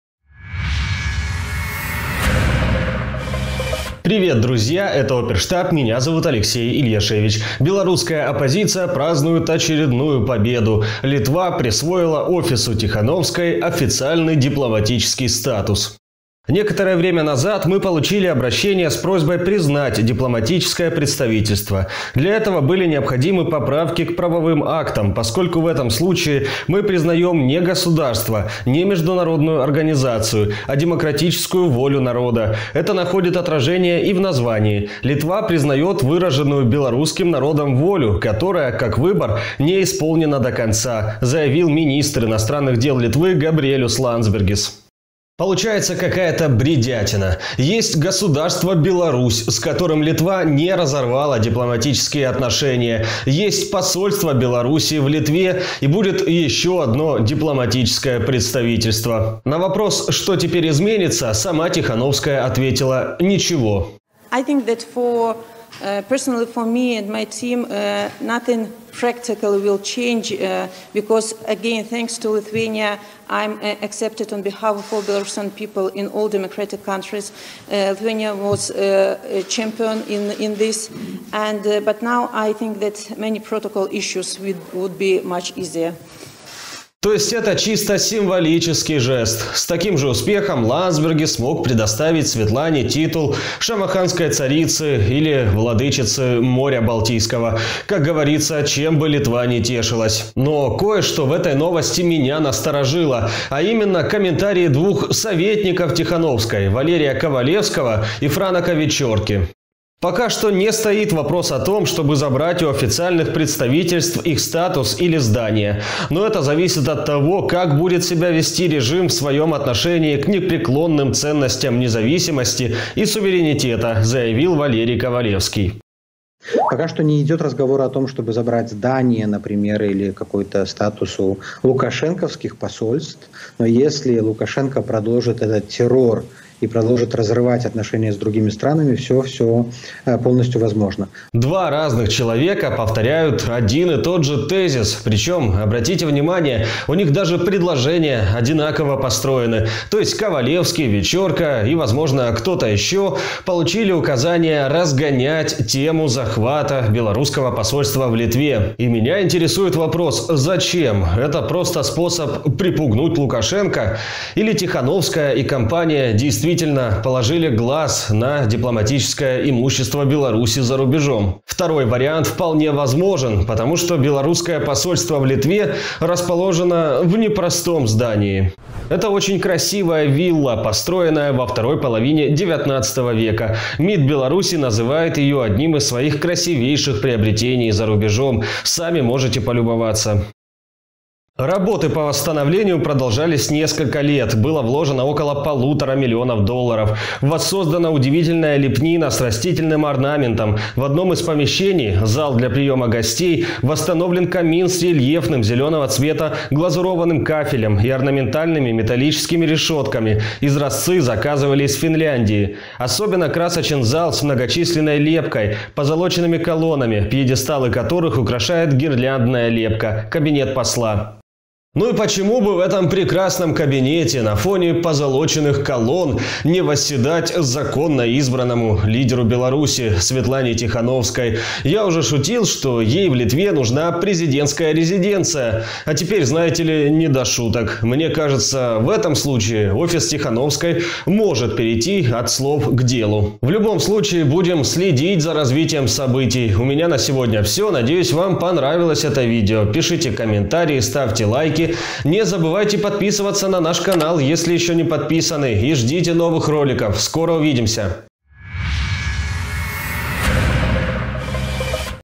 С ума сошла! Тихановская хочет захватить белорусское посольство в Литве
Белорусская оппозиция празднует очередную победу. Министерство иностранных дел Литвы предоставило офису Тихановской официальный дипломатический статус. Это чисто символический жест. С таким же успехом Ландсбергис мог предоставить Светлане титул Шамаханской царицы или владычицы моря Балтийского. Как говорится, чем бы Литва ни тешилась. Но кое-что в этой новости меня насторожило. А именно – комментарии двух советников Тихановской – Валерия Ковалевского и Франака Вячорки.

Обозреватель RuBaltic.Ru Алексей Ильяшевич — оперативно об актуальных событиях в Прибалтике и Восточной Европе в проекте «Оперштаб».